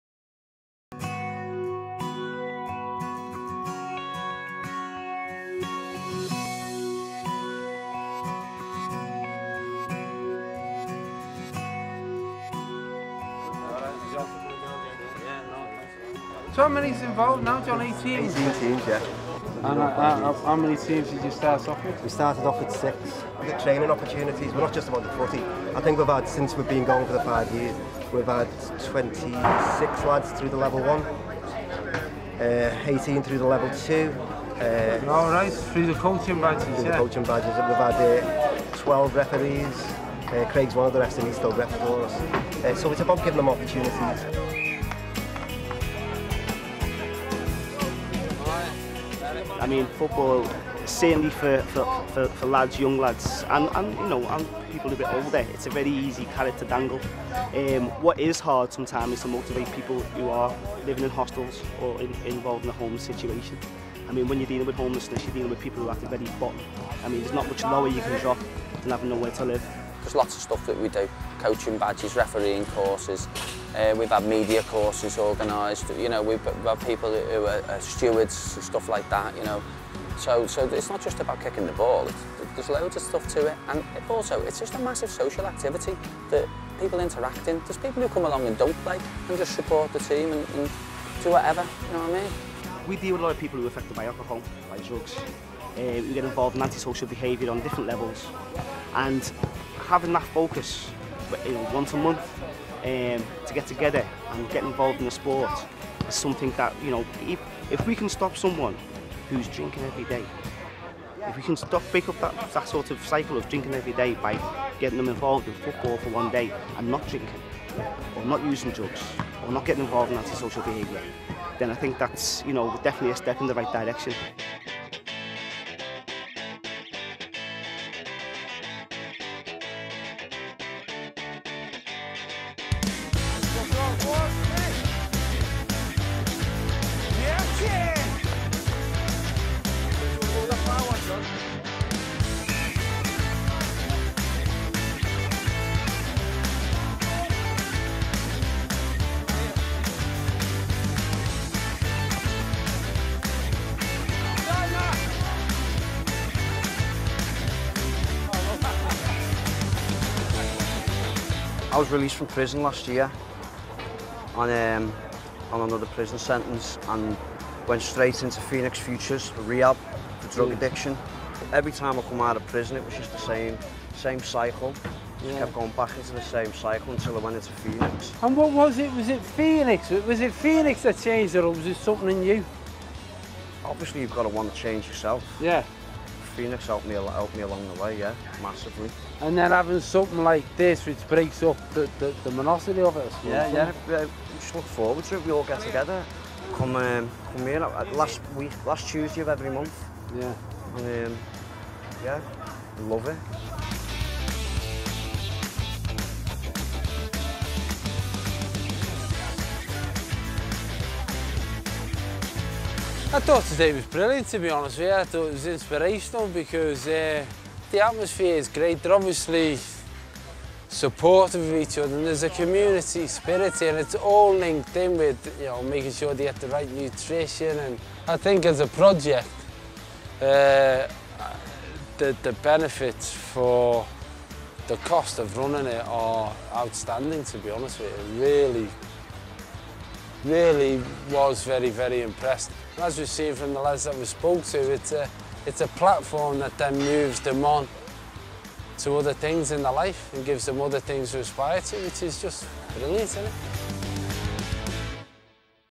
John Bishop Visit March 2012 by BBC Sport Relief
John Bishop's visit in March 2012 as part of his Sport Relief Campaign.
John was so impressed he has agreed to be our patron for the next 2 years.